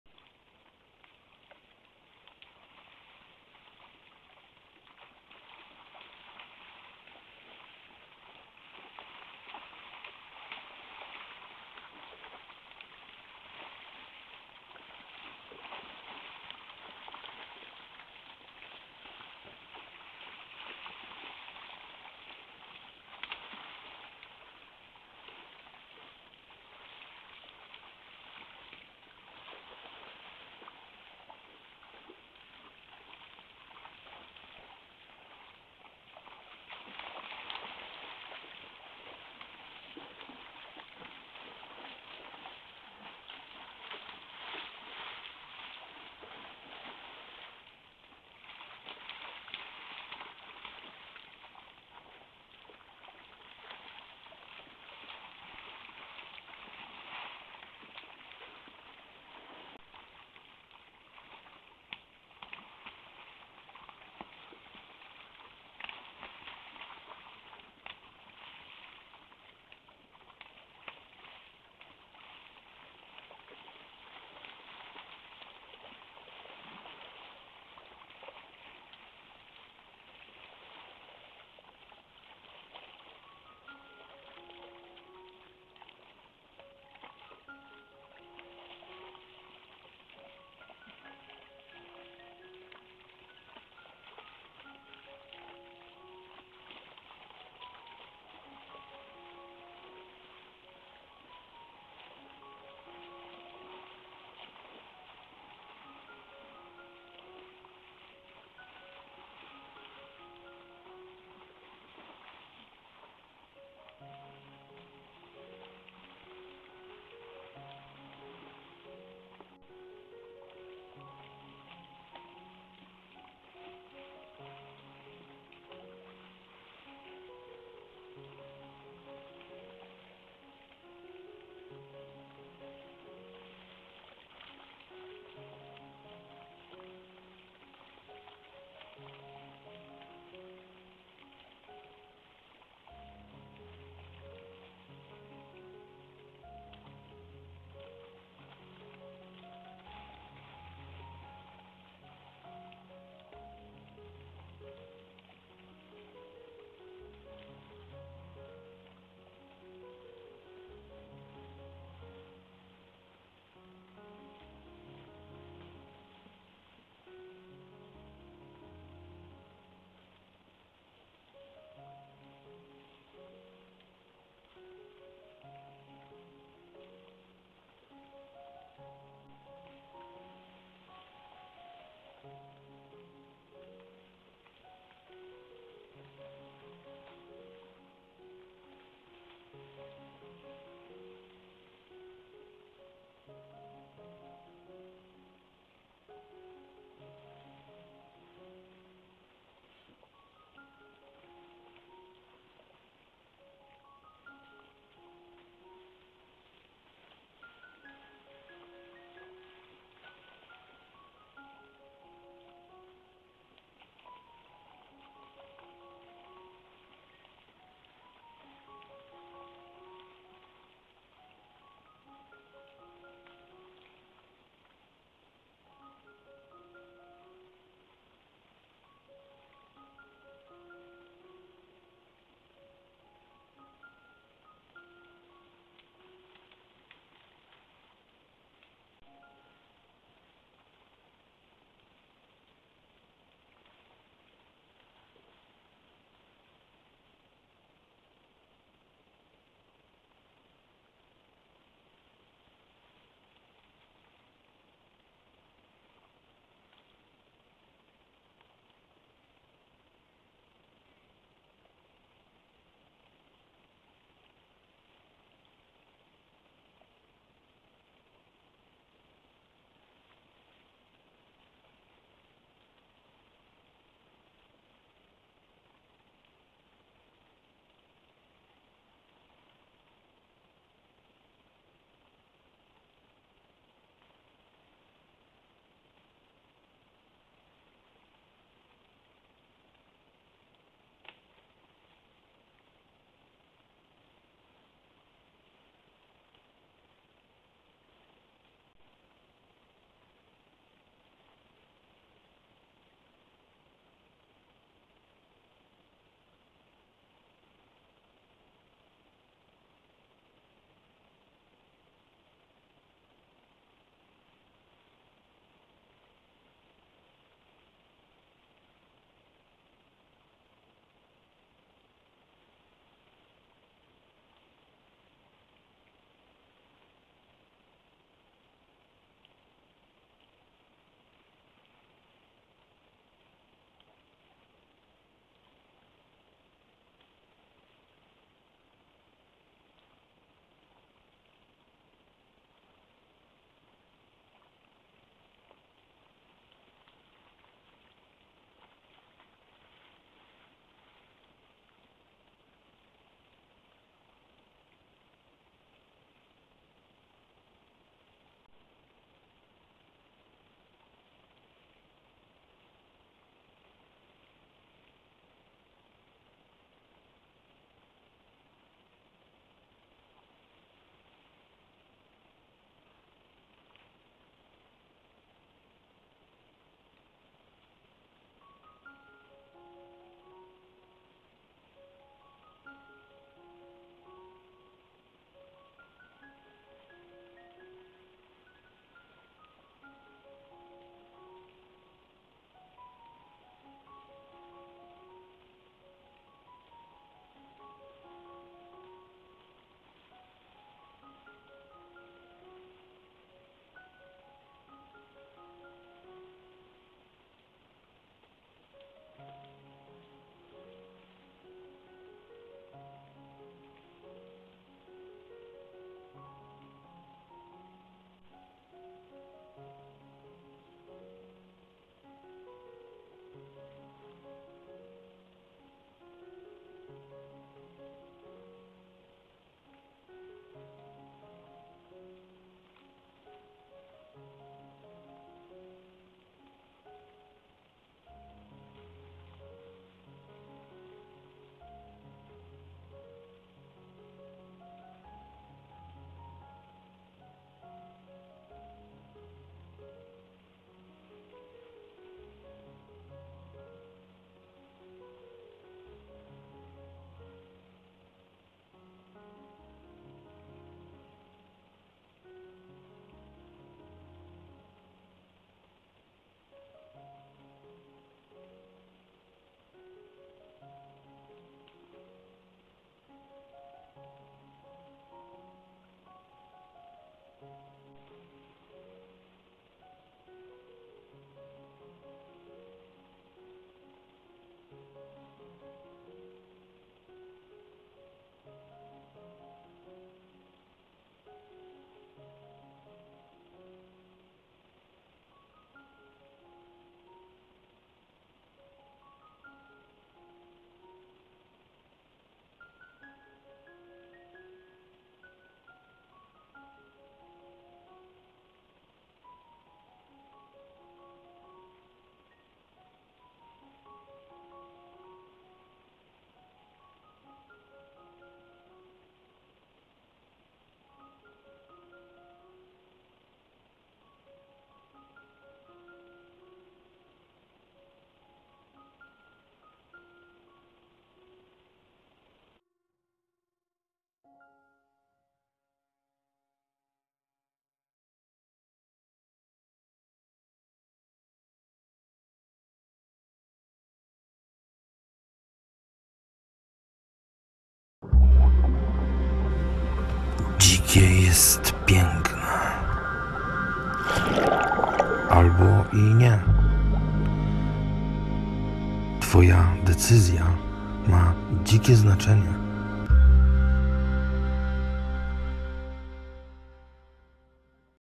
Chmara jeleni i byk przy kąpieli. Przepiękny kadr i naturalne zachowanie
- Niezwykłe nagranie zachowania chmary i relacji jeleni o zmroku
- Więcej filmów przyrodniczych w Playlista- PRZYRODA, NATURA, ZWIERZĘTA